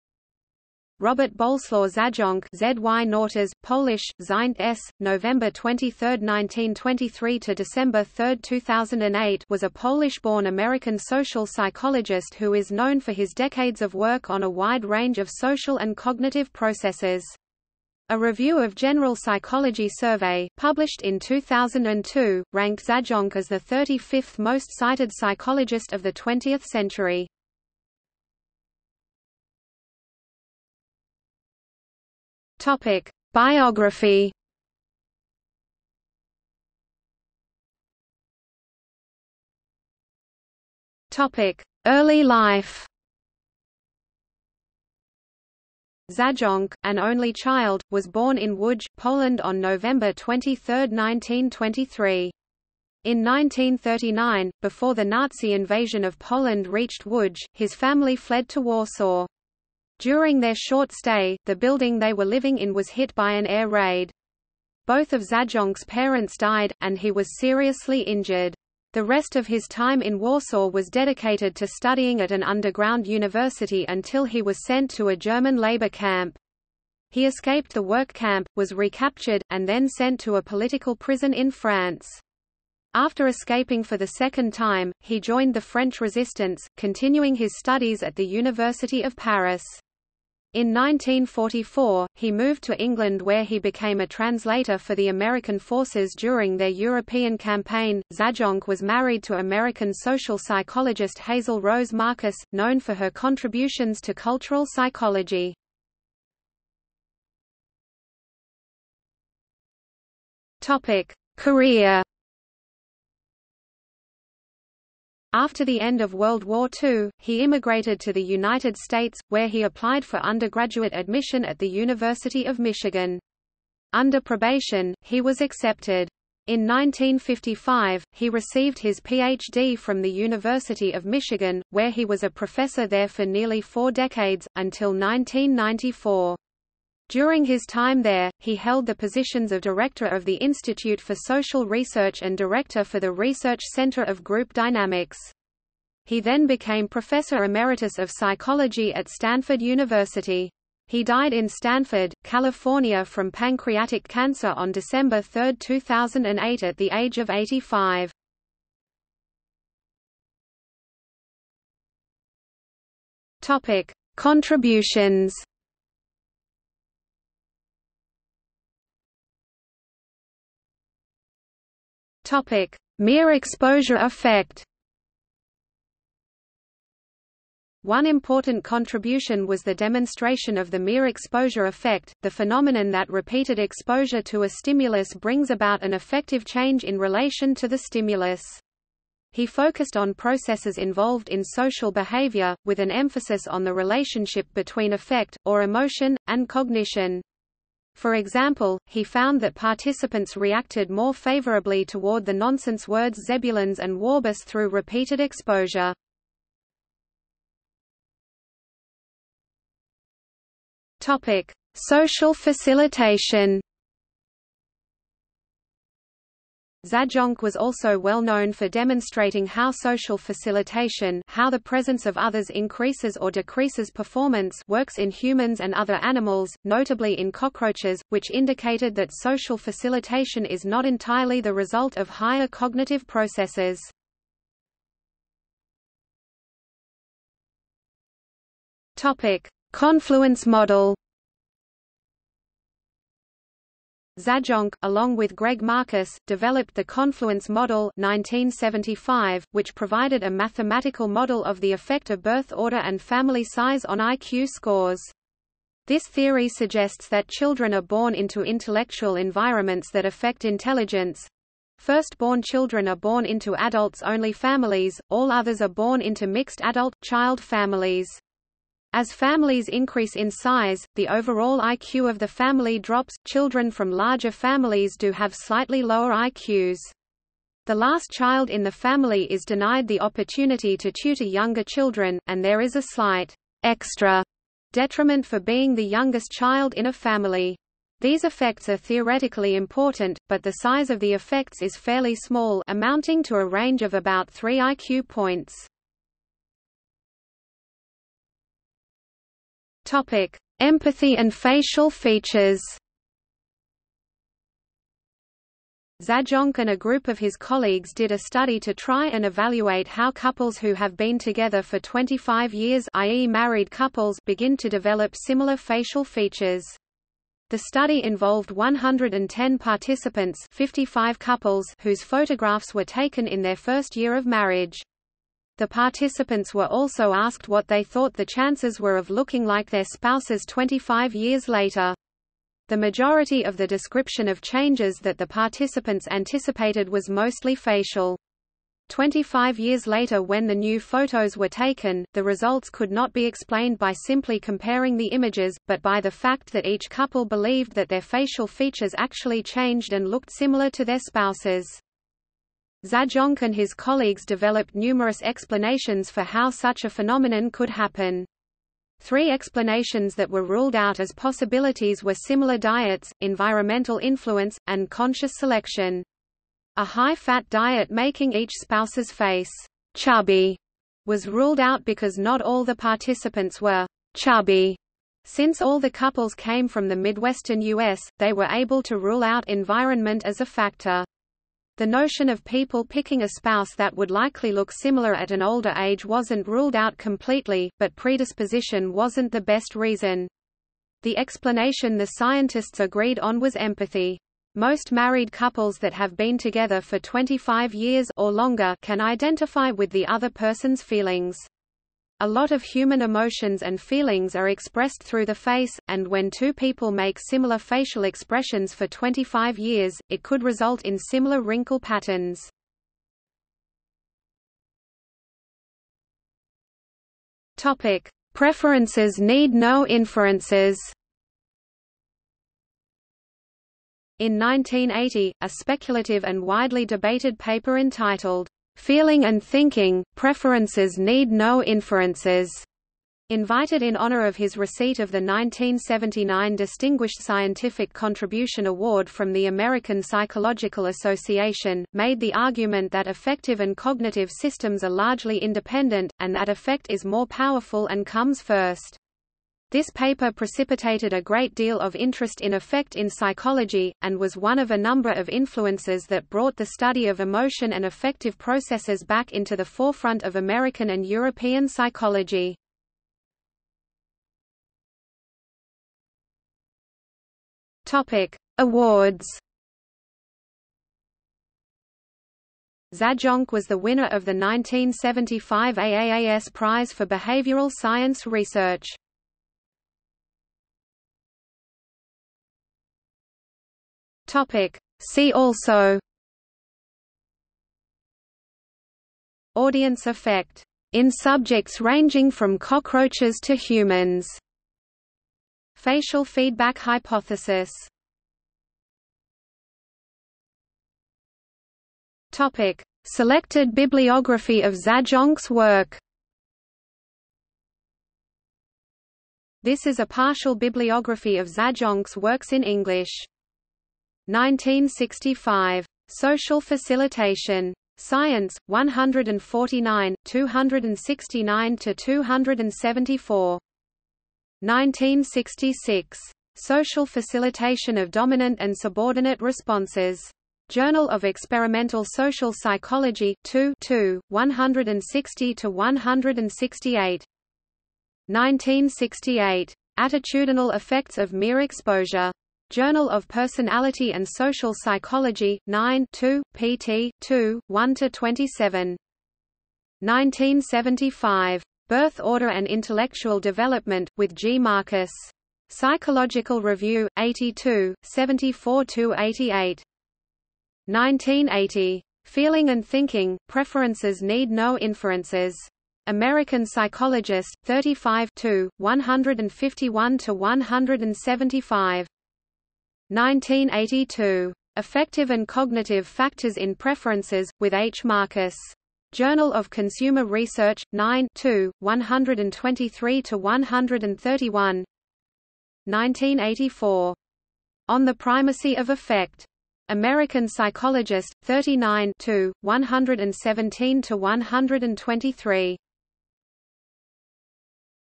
Robert Zajonc | Wikipedia audio article
00:00:29 1 Biography
00:00:37 1.1 Early life
00:01:47 1.2 Career
00:02:39 2 Contributions
00:02:49 2.1 Mere-exposure effect
00:03:27 2.2 Social facilitation
00:03:55 2.3 Confluence model
00:04:56 2.4 Empathy and facial features
00:06:54 2.5 Preferences need no inferences
00:07:44 3 Awards
00:08:00 4 See also
00:08:18 5 Selected bibliography of Zajonc's work

- Socrates

SUMMARY
Robert Bolesław Zajonc ( ZY-ənts; Polish: [ˈzajɔnt͡s]; November 23, 1923 – December 3, 2008) was a Polish-born American social psychologist who is known for his decades of work on a wide range of social and cognitive processes. A Review of General Psychology survey, published in 2002, ranked Zajonc as the 35th most cited psychologist of the 20th century.